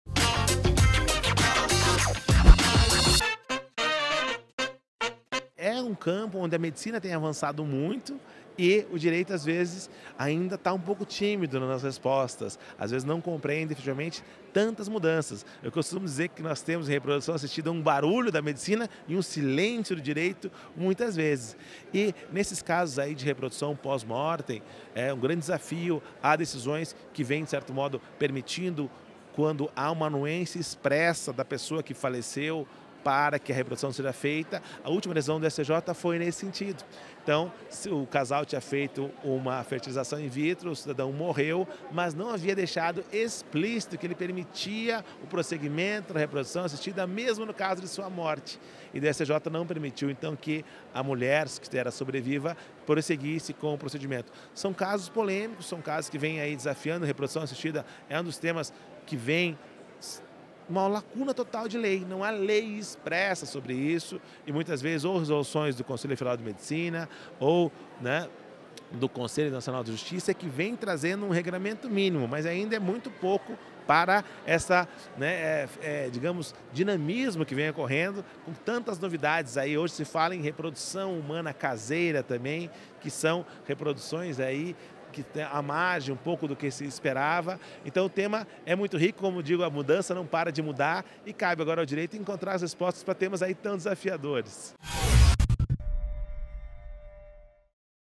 Ricardo Calderón sobre reprodução assistida: "Barulho da medicina, silêncio do Direito"
O tema da reprodução assistida ainda é um desafio para o Direito, destaca o advogado Ricardo Calderón, visto que há lacuna na lei sobre o tema.

O tema foi abordado em entrevista ao Migalhas durante o 13º Encontro Anual AASP.